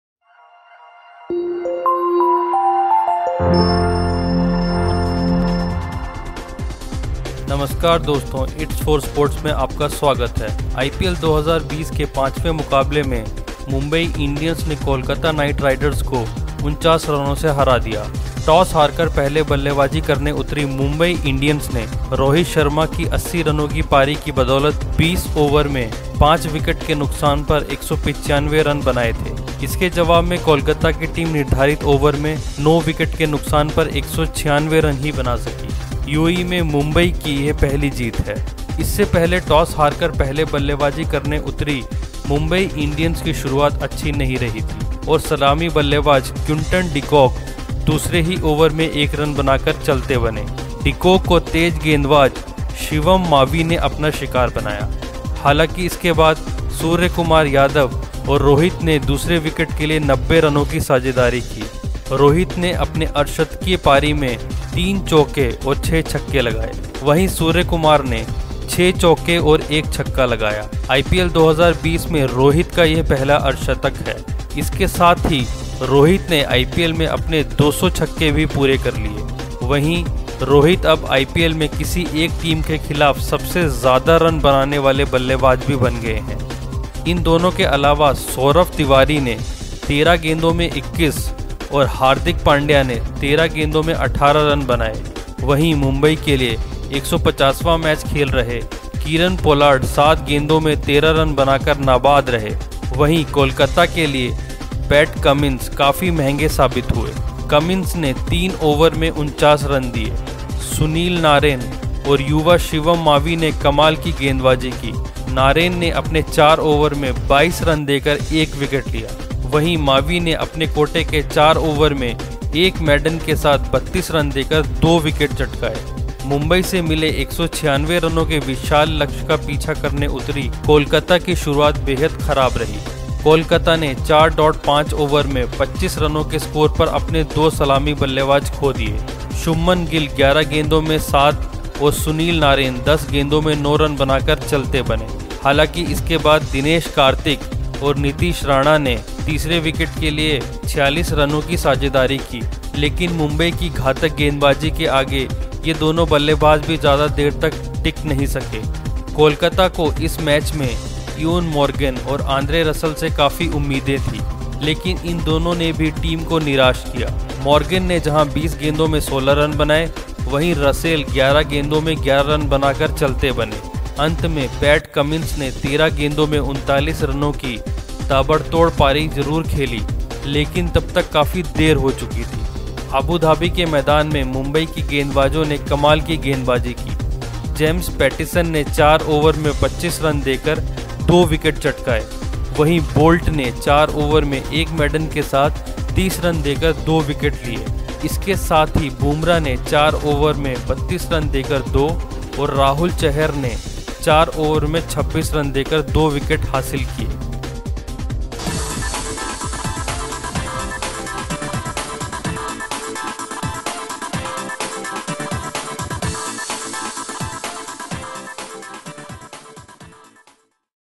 IPL 2020 KKR vs MI: यूएई में मुंबई ने दर्ज की पहली जीत, कोलकाता को 49 रनों से हराया
KKR vs MI: आईपीएल 2020 के पांचवें मुकाबले में मुंबई इंडियंस ने कोलकाता नाइट राइडर्स को 49 रनों से हरा दिया. टॉस हारकर पहले बल्लेबाज़ी करने उतरी मुंबई इंडियंस ने रोहित शर्मा की 80 रनों की पारी की बदौलत 20 ओवर में पांच विकेट के नुकसान पर 195 रन बनाए थे. इसके जवाब में कोलकाता की टीम निर्धारित ओवरों में नौ विकेट के नुकसान पर 146 रन ही बना सकी. यूएई में मुंबई की यह पहली जीत है.

इससे पहले टॉस हारकर पहले बल्लेबाज़ी करने उतरी मुंबई इंडियंस की शुरुआत अच्छी नहीं रही थी और सलामी बल्लेबाज़ क्विंटन डिकॉक दूसरे ही ओवर में एक रन बनाकर चलते बने. डिकॉक को तेज़ गेंदबाज़ शिवम मावी ने अपना शिकार बनाया. हालांकि, इसके बाद सूर्यकुमार यादव 47 और रोहित 80 ने दूसरे विकेट के लिए 90 रनों की साझेदारी की.